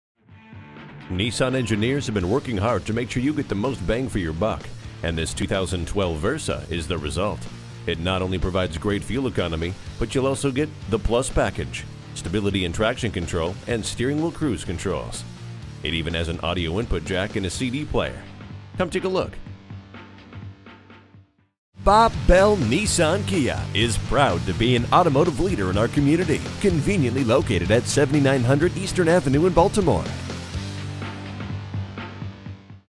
2012 Nissan Versa Baltimore MD Dundalk, MD N50124 - SOLD
Year: 2012
Make: Nissan
Model: Versa
Trim: 1.8 S
Engine: 1.8L 4 cyls
Transmission: Automatic 4-Speed
Color: Blue Onyx Metallic
Interior: Gray
Stock #: N50124
This gas-saving Vehicle will get you where you need to go** Safety Features Include: ABS Traction control Curtain airbags Passenger Airbag Stability control...

7900 Eastern Ave.

Address:
7900 Eastern Ave